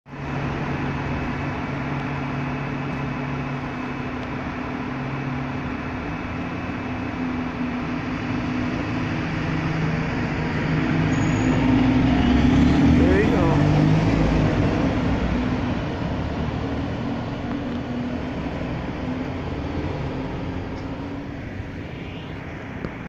Wigan bus catch just by the train station
I had a visit Wigan it's the first major train station south of Preston, in the past when travelling back from Blackpool the would pass over Wigan station and I would see the buses, so I'm here now